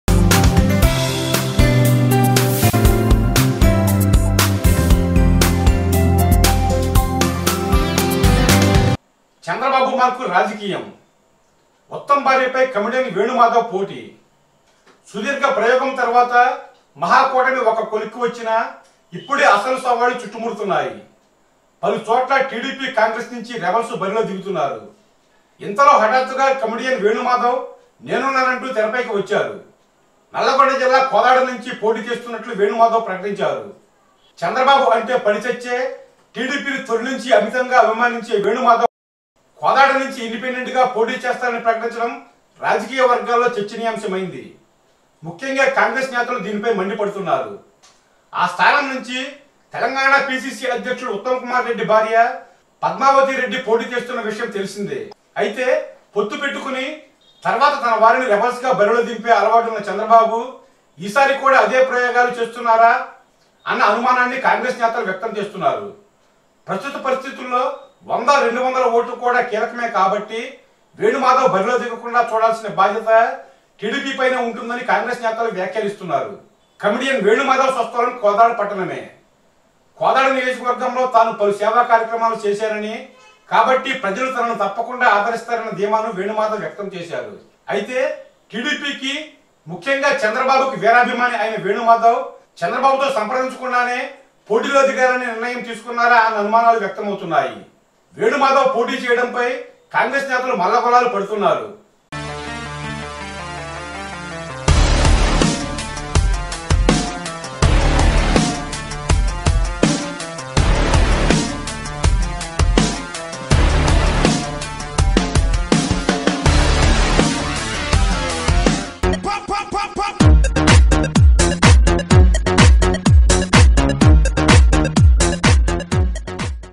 ఉత్తమ్ భార్యపై వేణుమాధవ్ పోటీ.. చంద్రబాబు వ్యూహమేనా?| Comedian Venumadhav to Contest from Kodad..!
Tollywood Comedian Venumadhav is TDP Activist and a big fan of AP CM Chandrababu Naidu. Now he preparing to contest in Telangana elections from his own town Kodad as an Independent Candidate. We know the TPCC President Uttam Kumar Reddy wife Padmavathi Reddy is decided to contest from that Constituency as Congress Candidate. Congress leaders are shocked with the Venu Madhav decision.
Watch this video to know the Chandrababu Naidu plan behind Venu Madhav Contesting.